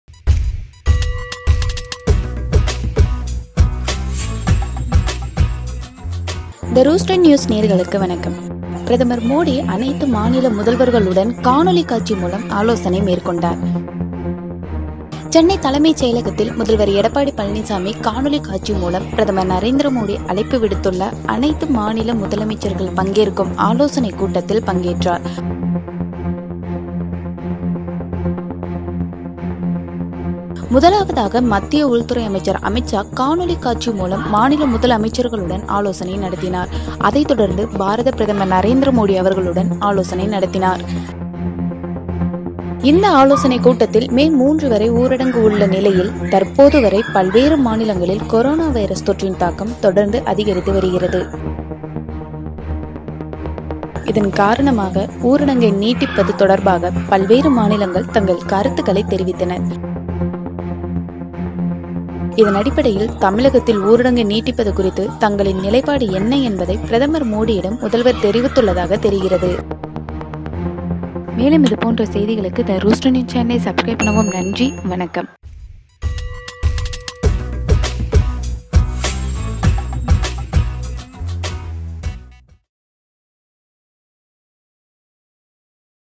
மே 3-க்கு பிறகும் பொது முடக்கம் நீடிக்கும் | Pm Modi Video Conference Meeting With Chief Ministers
கொரோனா அதிகமான இடங்களில் மே 3- க்கு பிறகும் பொது முடக்கம் நீடிக்கும் | மோடி ஆலோசனை | Pm Narendra Modi Video Conference Meeting With Chief Ministers

 PM Modi : ஊரடங்கை தளர்த்துவது குறித்து, அனைத்து மாநில முதல்வர்களுடன் ஆலோசனை நடத்தினார் பிரதமர் மோடி.

சென்னை தலைமைச்செயலகத்தில் முதல்வர் எடப்பாடி பழனிச்சாமி காணொளி காட்சி மூலம் பிரதமர் நரேந்திர மோடி அழைப்பு விடுத்துள்ள அனைத்து மாநில முதலமைச்சர்கள் பங்கேற்கும் ஆலோசனைக் கூட்டத்தில் பங்கேற்றார்.

முதலாவதாக மத்திய உள்துறை அமைச்சர் அமித்ஷா காணொளி காட்சி மூலம் மாநில முதலமைச்சர்களுடன் ஆலோசனை நடத்தினார் அதைத்தொடர்ந்து பாரத பிரதமர் நரேந்திரமோடி அவர்களுடன் ஆலோசனை நடத்தினார்..

இந்த ஆலோசனைக் கூட்டத்தில்   மே3 வரை ஊரடங்கு உள்ள நிலையில் தற்போது வரை பல்வேறு மாநிலங்களில் கொரோனா வைரஸ் தொற்றின் தாக்கம் தொடர்ந்து அதிகரித்து வருகிறது. இதன் காரணமாக ஊரடங்கை நீட்டிப்பதுத் தொடர்பாக பல்வேறு மாநிலங்கள் தங்கள் கருத்துக்களைத் தெரிவித்தனர்.

இதனடிப்படையில்  தமிழகத்தில் ஊரடங்கு நீட்டிப்பது குறித்து தங்களின் நிலைபாடு என்ன என்பதை  பிரதமர் மோடியிடம் முதல்வர் தெரிவித்துள்ளதாக தெரிகிறது.